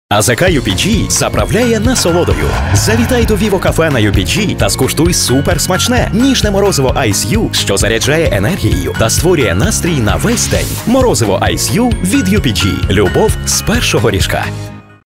Морозиво ice U від АЗК UPG
Морозиво ice U – ще одна чудова нагода завітати на АЗК UPG.
Кожен знайде своє ідеальне поєднання смаків і текстур. Та, як би довго ми не розповідали про насолоду, яку дарує наш прохолодний десерт у спекотний день, ви ж знаєте: краще один раз спробувати і, впевнені, прикипіти серцем назавжди.